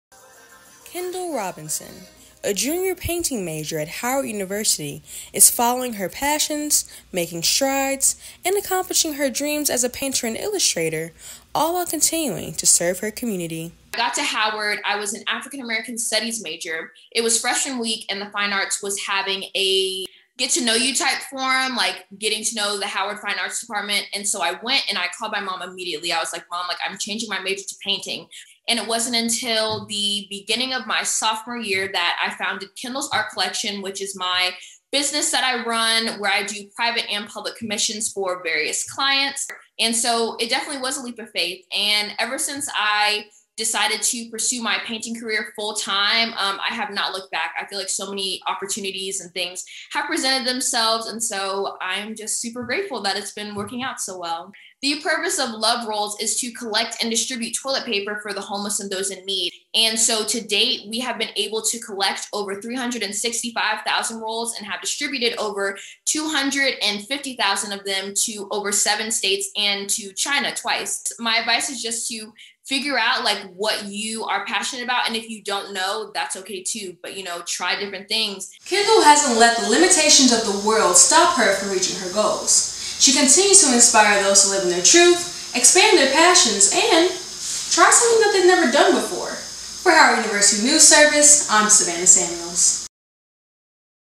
Creating Her Own Canvas: Howard University Painter Turns Passion Into Positivity
A Howard University Fine Arts major is making moves inside and outside of the classroom. She's a painter, illustrator and non-profit creator who leads with passion and purpose. NewsVision's Savanna Samuels reports.